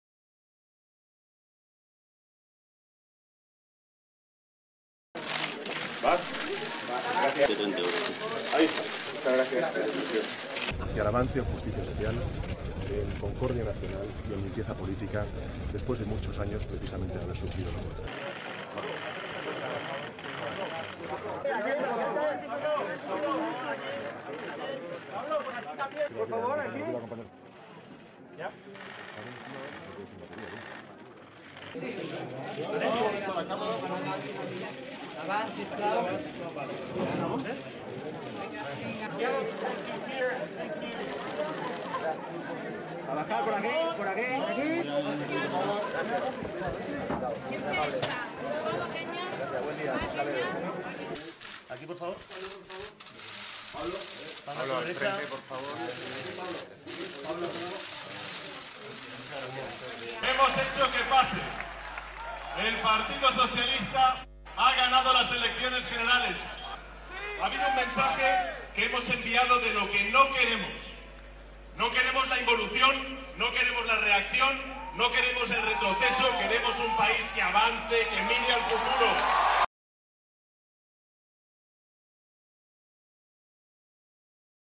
Los socialistas se imponen ante la derecha en España
El domingo 28 de abril de 2019, en las Elecciones Generales en España, el Partido Socialista Obrero Español ganó con una amplia ventaja sobre el Partido Popular, Ciudadanos y VOX.

El PSOE obtuvo 123 curules parlamentarios, mientras que el PP obtuvo sólo 66, convirtiéndola en una derrota histórica para el partido de derecha ibérico.